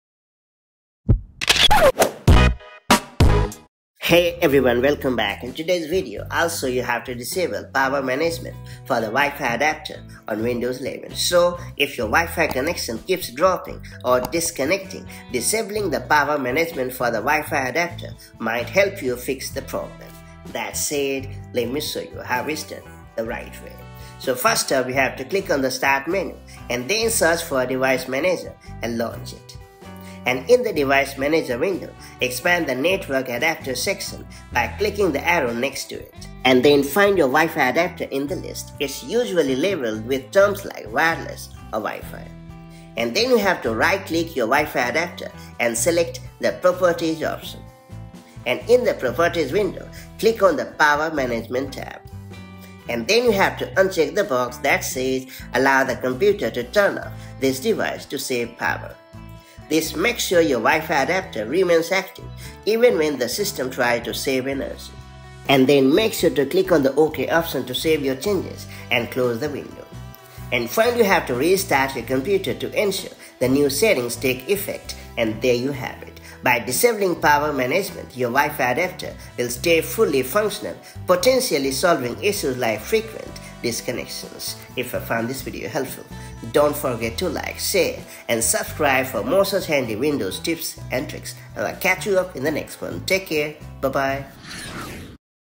How to Disable Power Management for the Wi-Fi Adapter on Windows 11
Are you facing connectivity issues or slow Wi-Fi on Windows 11? Disabling power management for the Wi-Fi adapter might help! In this video, we show you how to ensure your Wi-Fi stays consistently connected by tweaking the power settings. Follow along for a quick and effective fix to keep your internet connection stable.